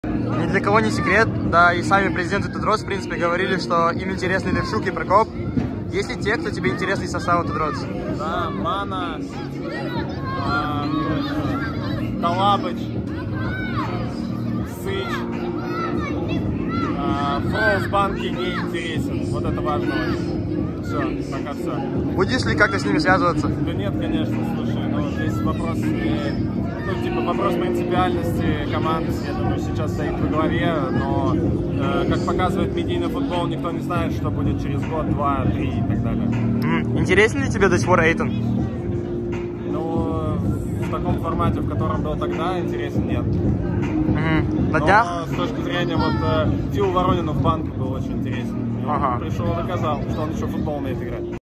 😳«МНЕ ИНТЕРЕСНЫ МАННА, ТАЛАБКО, СЫЧЕВ» — ГЕРМАН АМКАЛ
❗️❗️ПРЕЗИДЕНТ МФЛ РАССКАЗАЛ О ПРИЕЗДЕ РОНАЛДУ НА ЧЕМПИОНАТ МИРА ПО МЕДИАФУТБОЛУ
СВЕЖЕЕ ВЬЮ